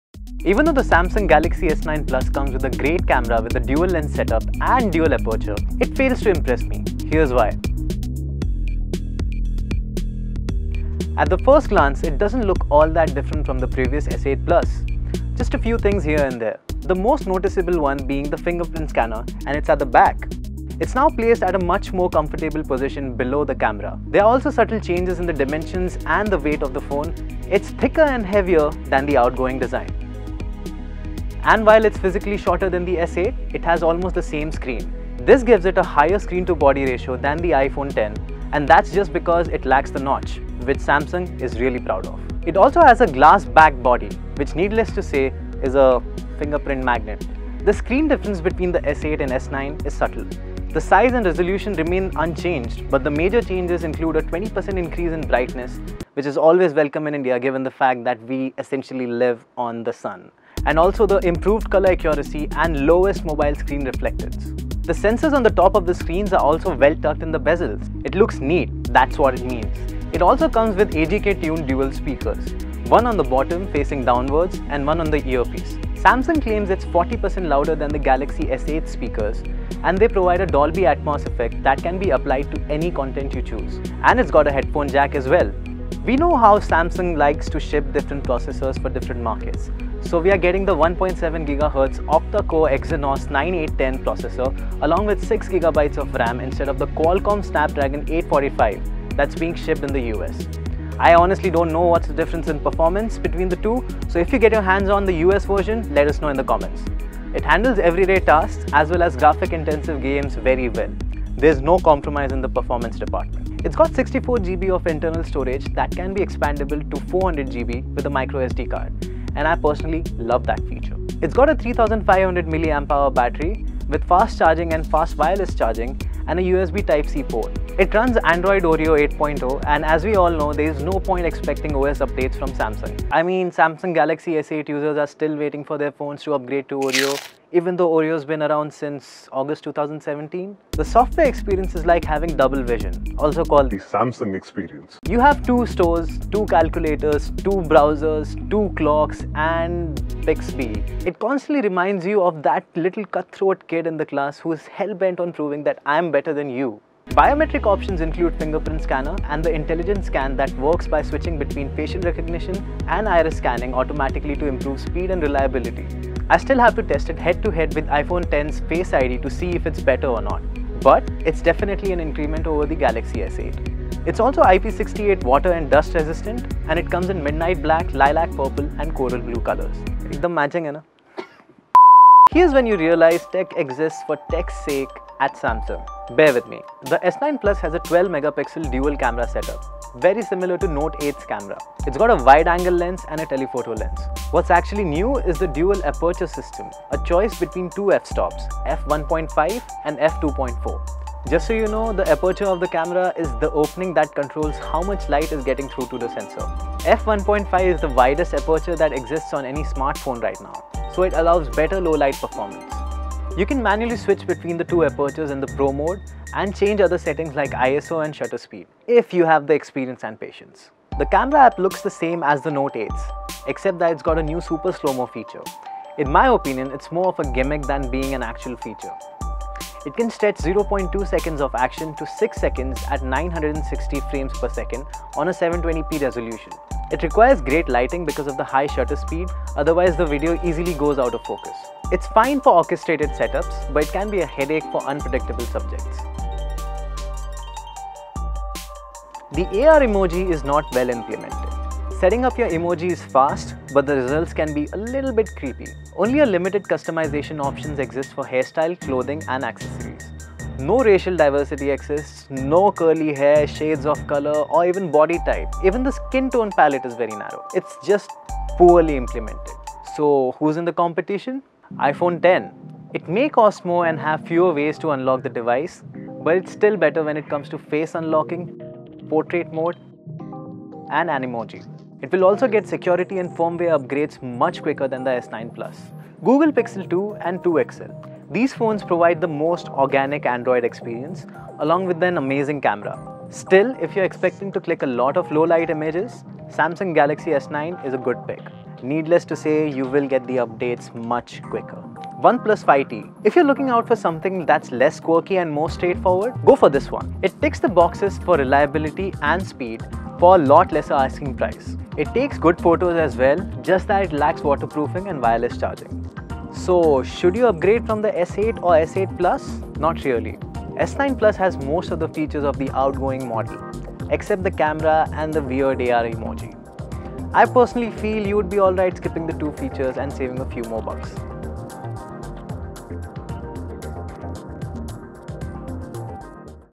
Tech It And Go: Samsung Galaxy S9 Plus doesn't impress. Here's why.
The Samsung Galaxy S9 looks incredible on the paper, but is it really all that great?

It was announced at the MWC 2018, Barcelona.

It's priced at Rs 64,900 and features a 6.2-inch Super AMOLED display with a 12-megapixel dual camera setup and a dual aperture system.
It has also kept up with the trend of Animojis set by iPhone X by coming up with its own variant of it - the ARemoji
It also has a super slo-mo feature that stretches out 0.2 seconds of action to 6 seconds.
It’s also IP68 water and dust resistant and comes in midnight black, lilac purple, and coral blue colors.

Biometric options include a fingerprint scanner and intelligent scan that works by switching between facial recognition and iris scanning automatically to improve speed and reliability.

iPhone X, Google Pixel 2 and 2 XL and OnePlus 5T all stand against the phone, but does the Galaxy S9 Plus take the cake?